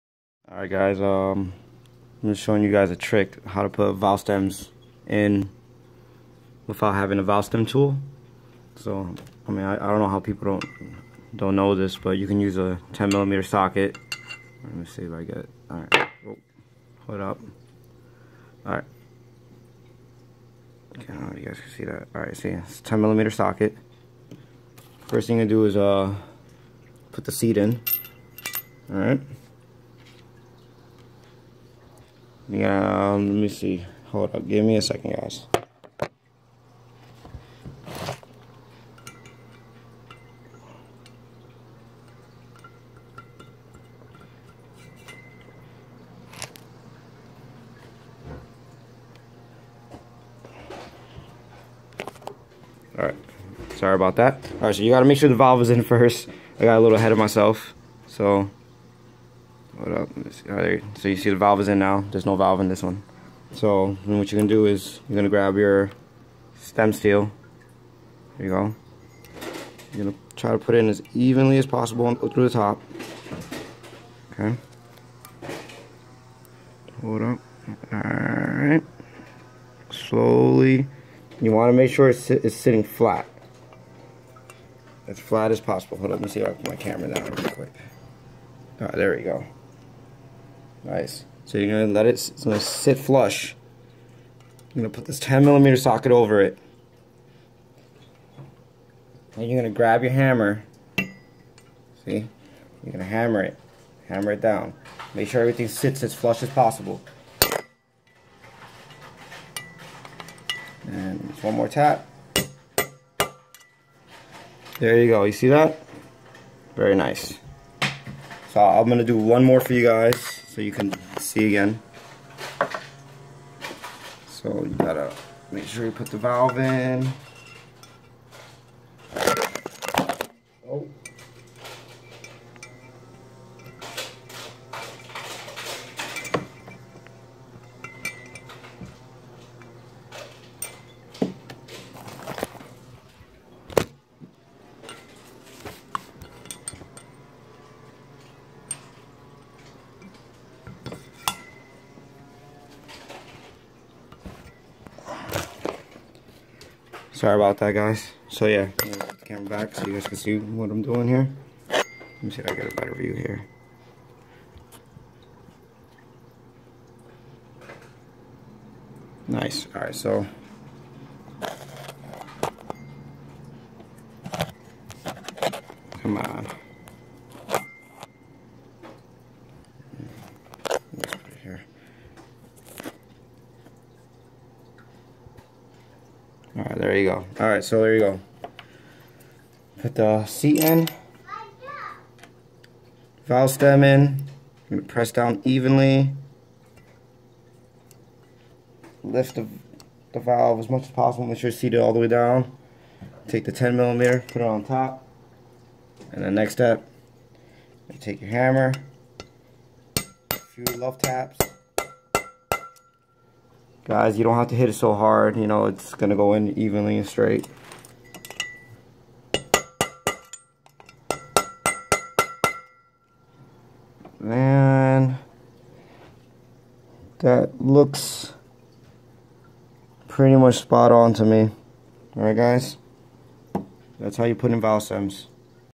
Valve stem seals k20 head
Just showing u guys simple trick to install valve stem seals with out a special tool.